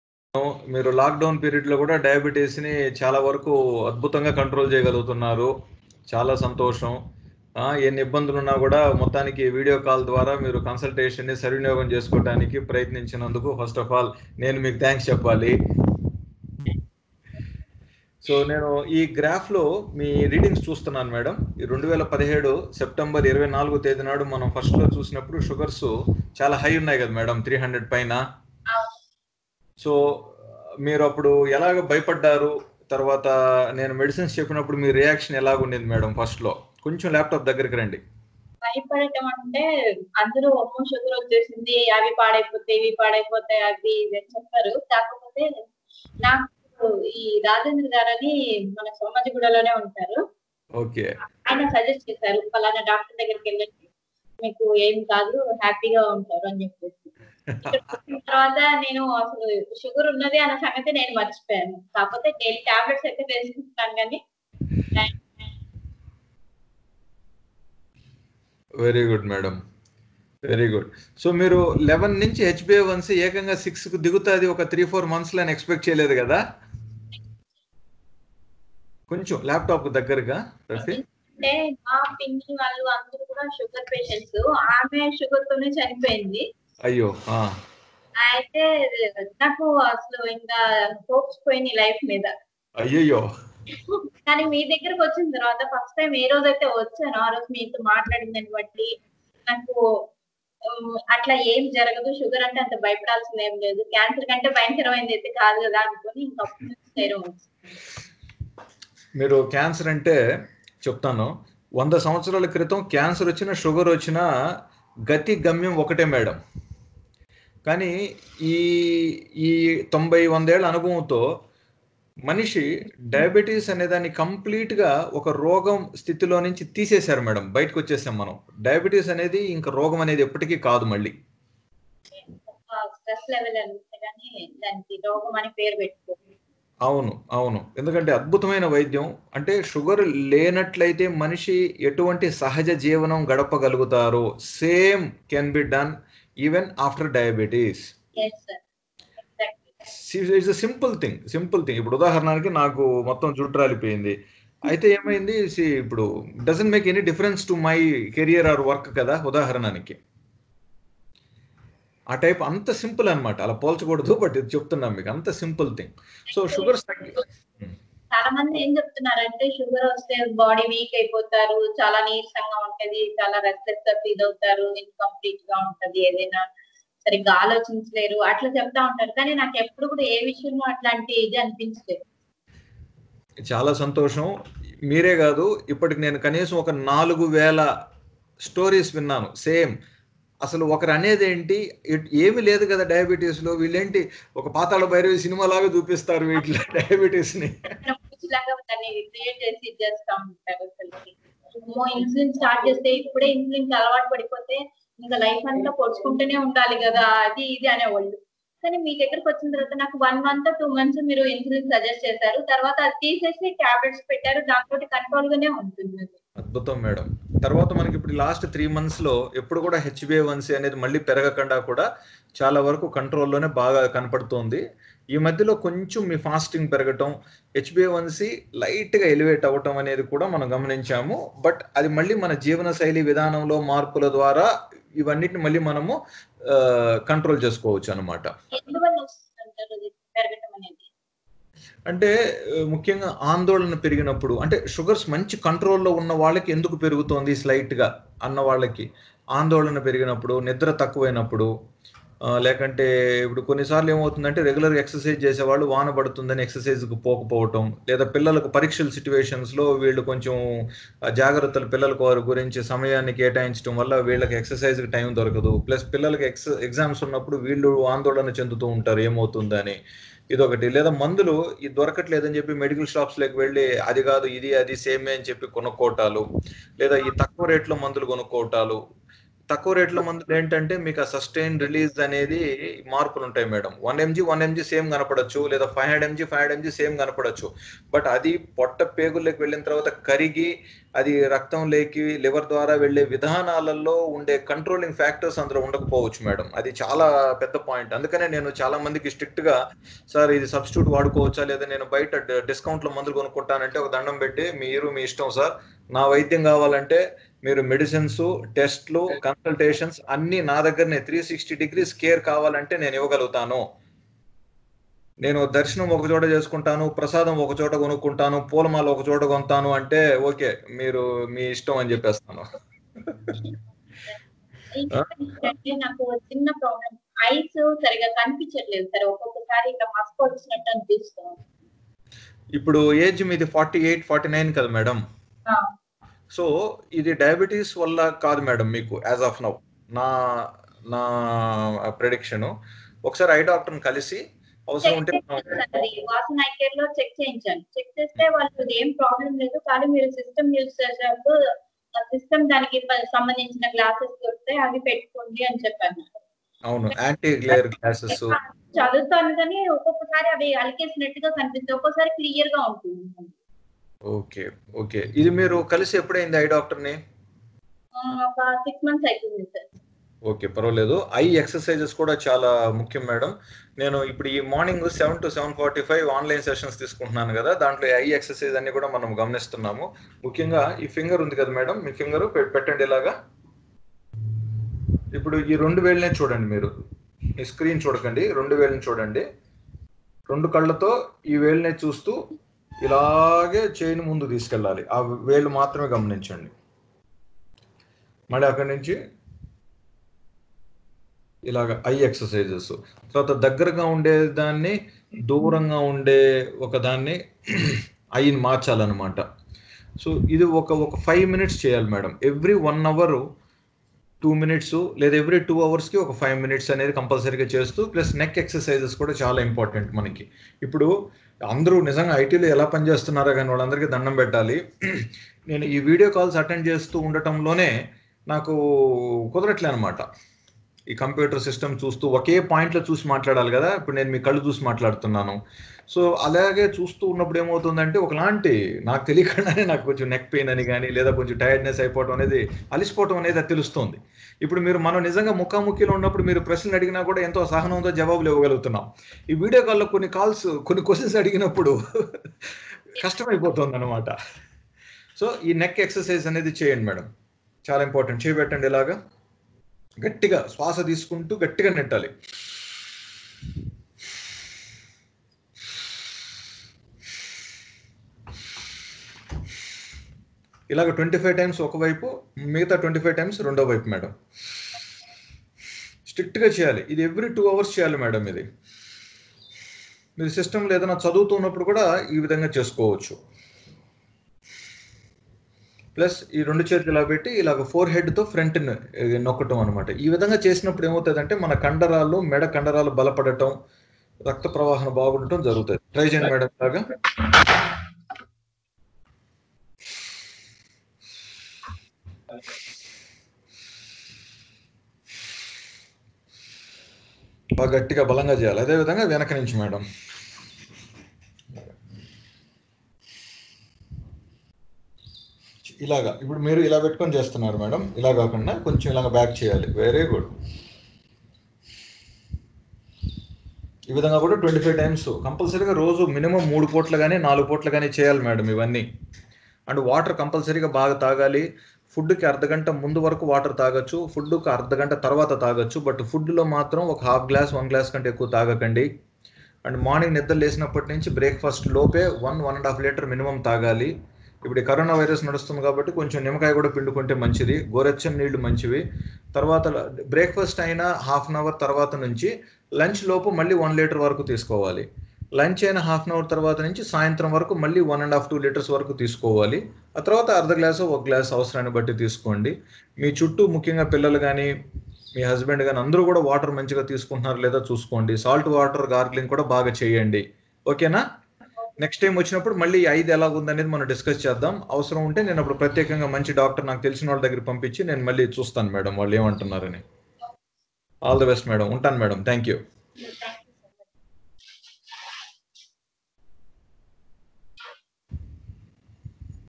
Telemedicine - online treatment Diabetes treatment .
video & audio along with a graphical representation of past history at Dr Kiran Diabetes centre.